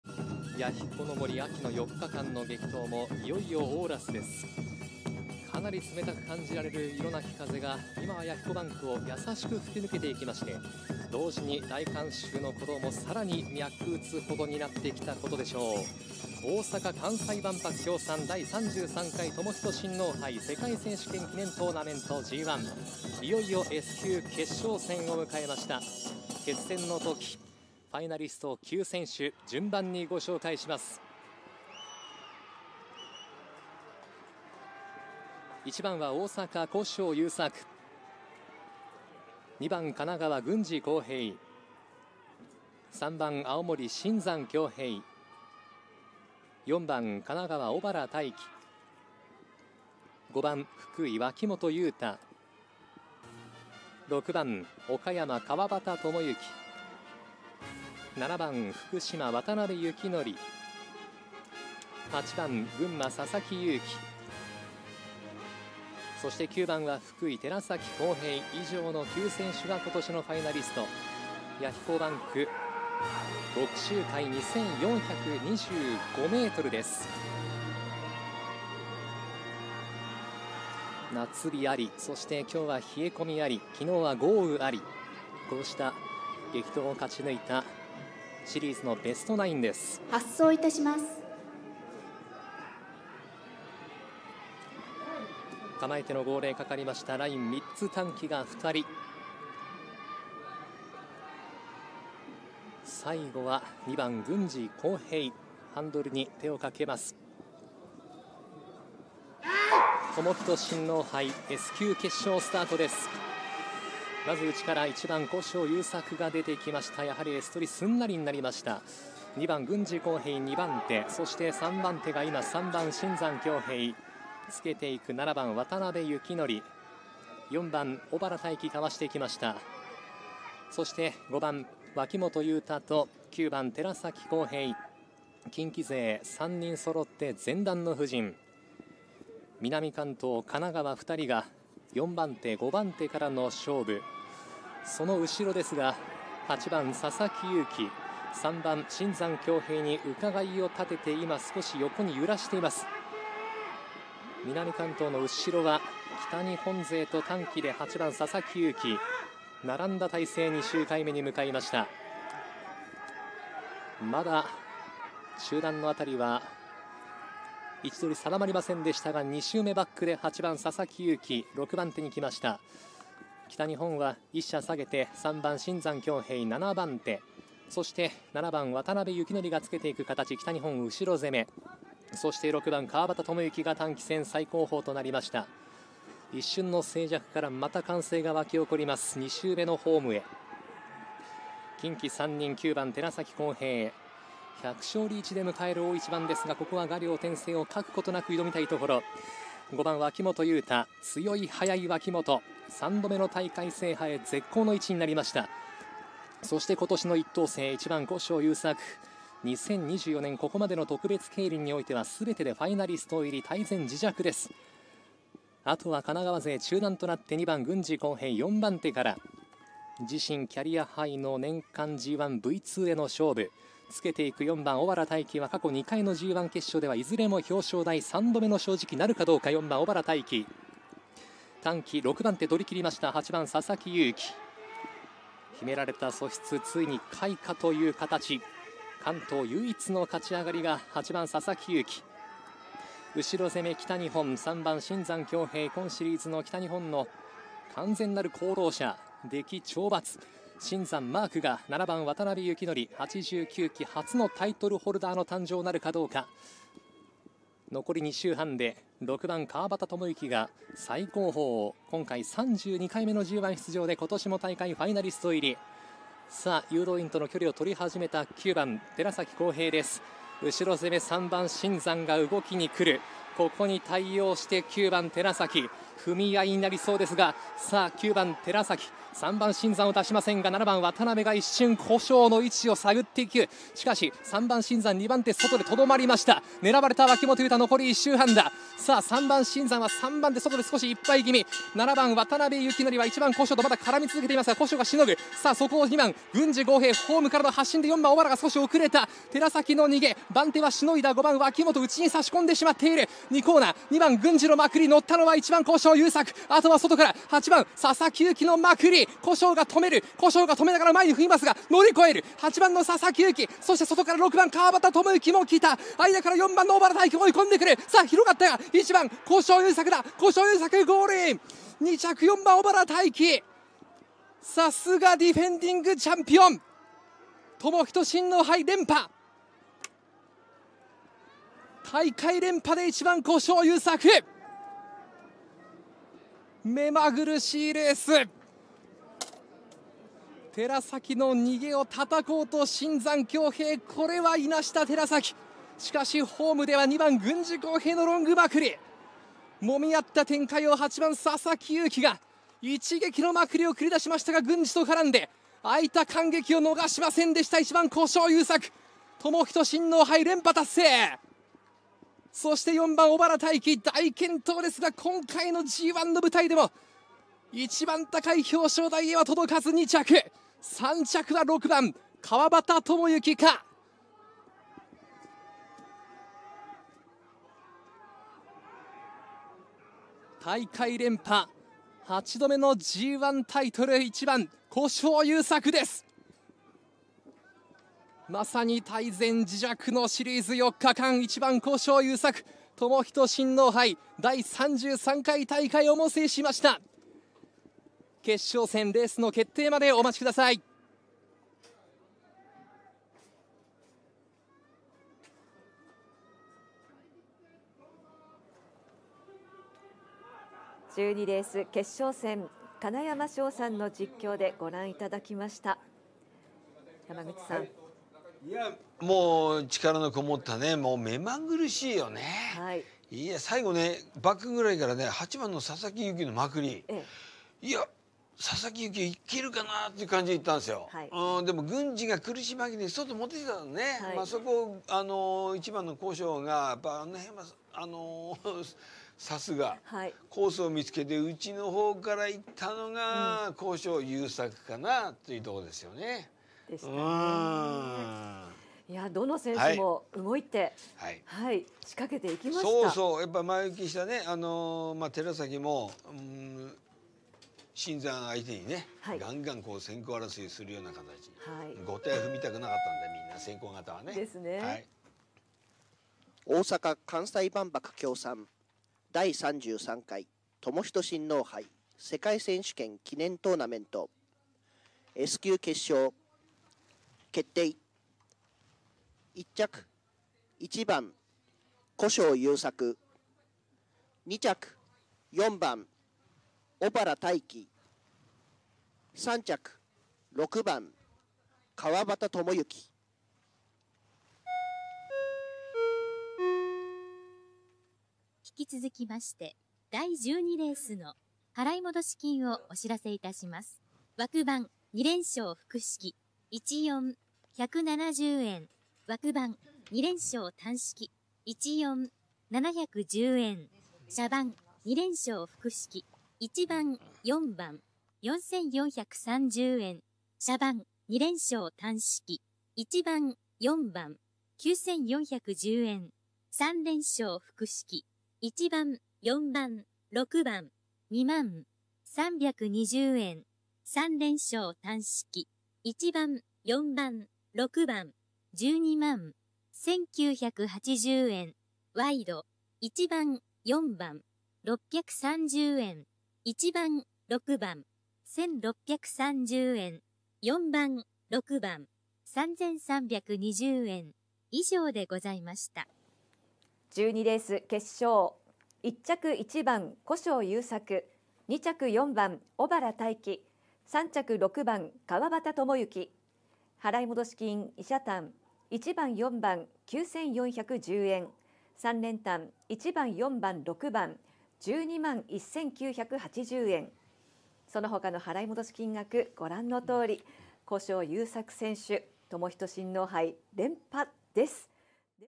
第33回寛仁親王牌Ｇ1 決勝
弥彦競輪場で開催されたＧ1寛仁親王牌決勝戦は古性選手の連覇となりました。おめでとう㊗️ごさいます。

３番手より落ち着いて瞬時に切り替えていい位置がとれる自在な選手ですね👍いつも落ち着いて見れます。